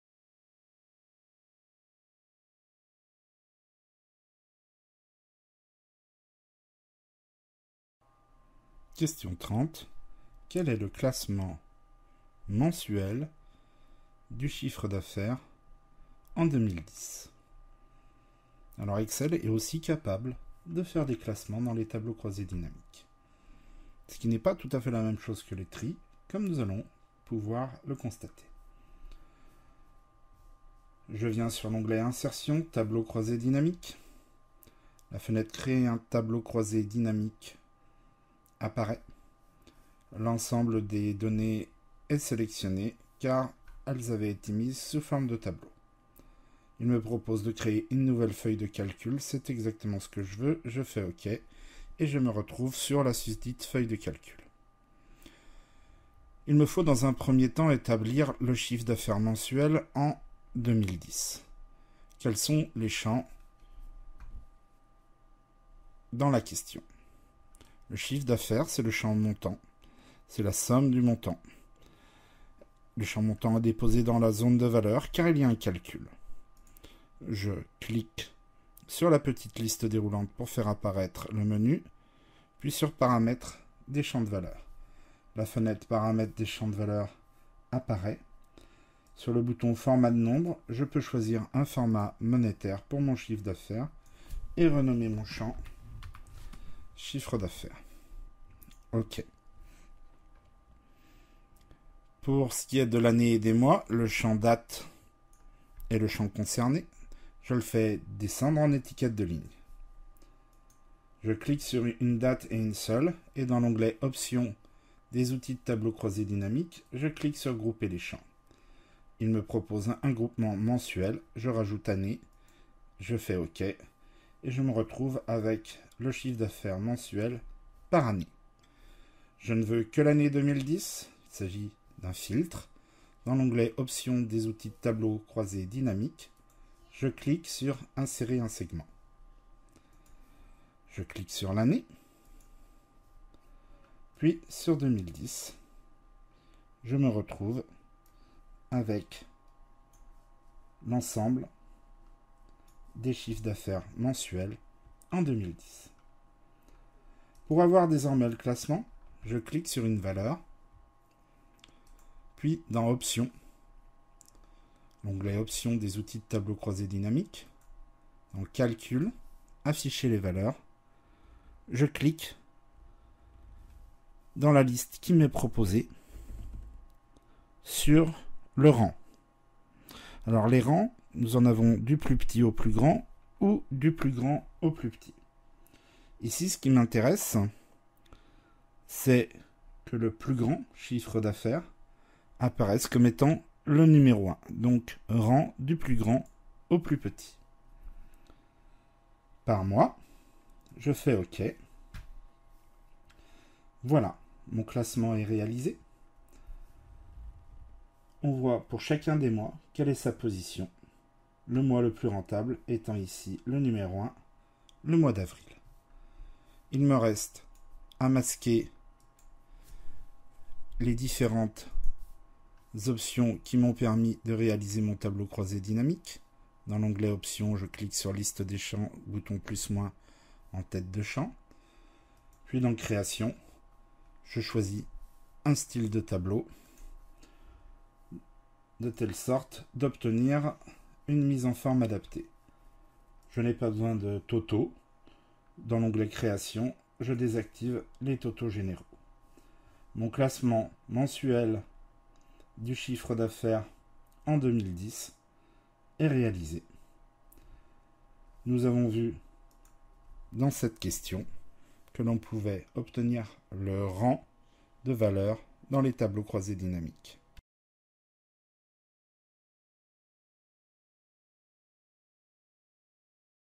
Les Tableaux croisés dynamiques (Exercice 30) - Formation Excel Marseille
Correction de la question 30 de l'exercice "Découverte des tableaux croisés dynamiques" .
Issu de la formation Excel de Optivalys Formations à Marseille.